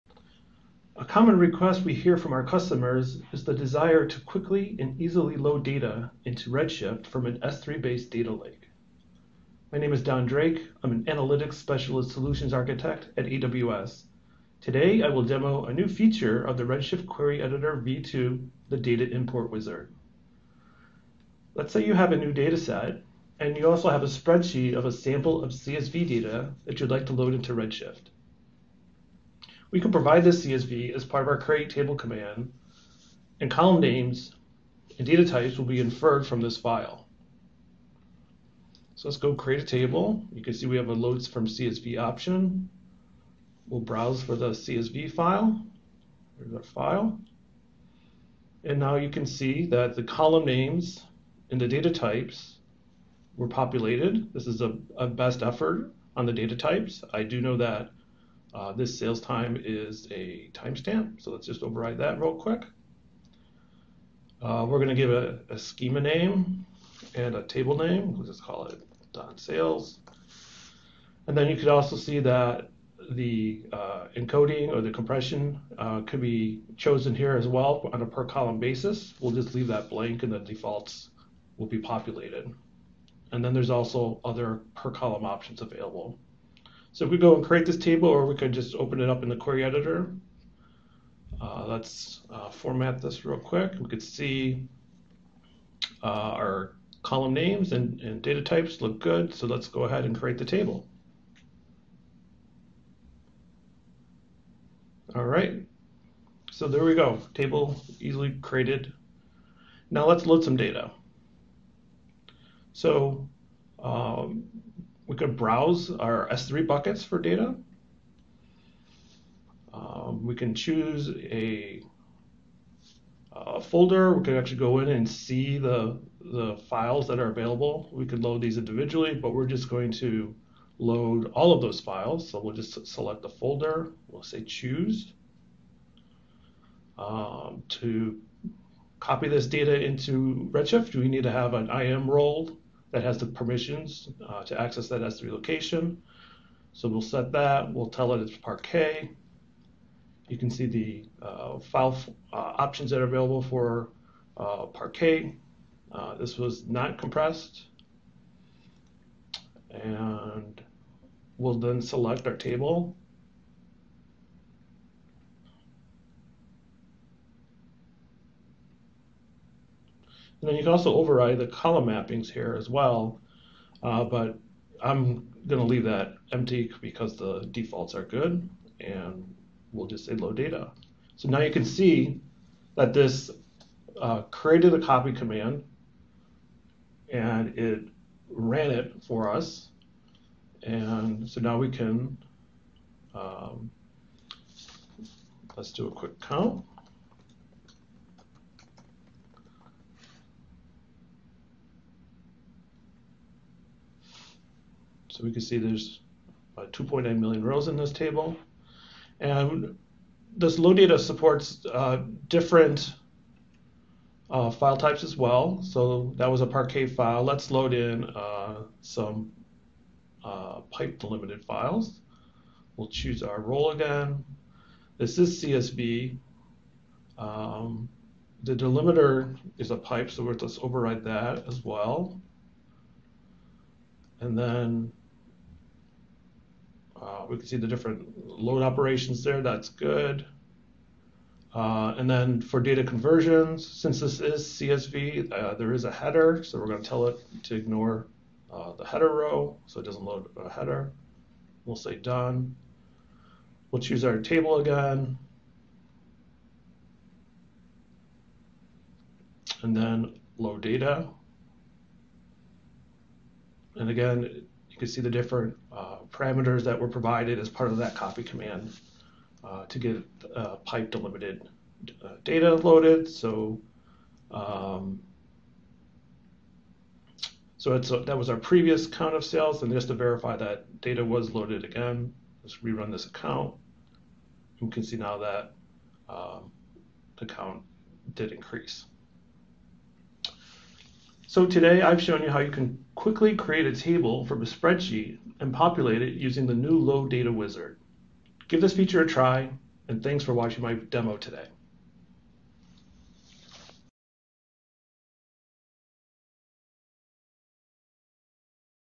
Data Loading with Query Editor v2 | Amazon Web Services
This is a demo of the new "Load data" wizard available in the Redshift Query Editor v2. A user can browse S3 locations for datasets and load them into Amazon Redshift tables without writing any code.